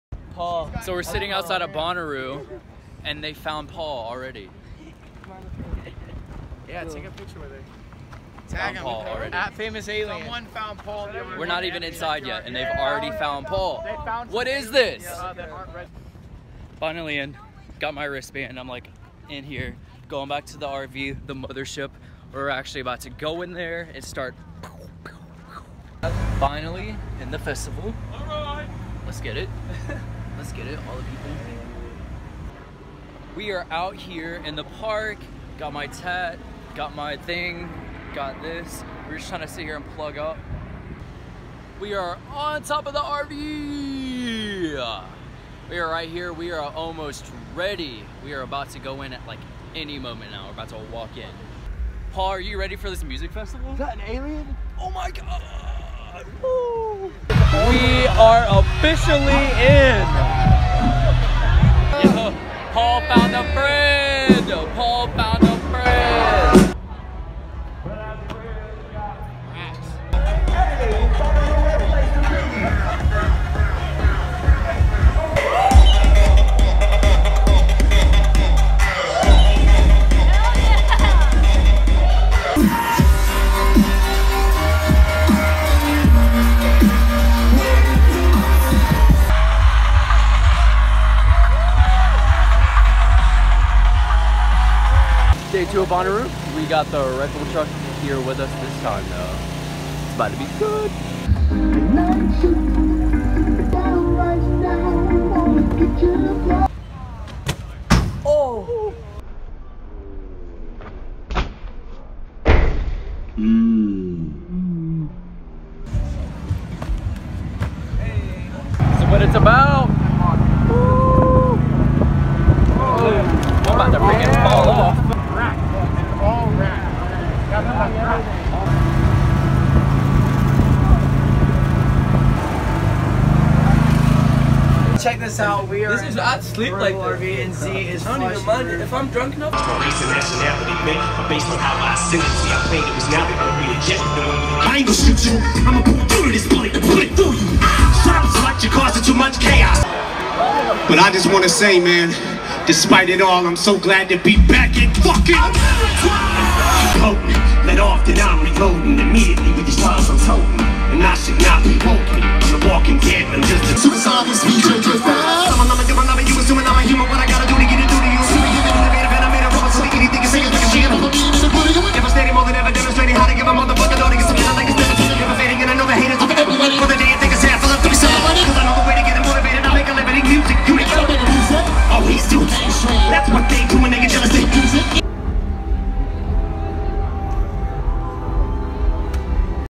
WE BROUGHT AN ALIEN TO BONNAROO
No words for this crazy weekend! If you have never been to Bonnaroo it is a must for the bucket list!!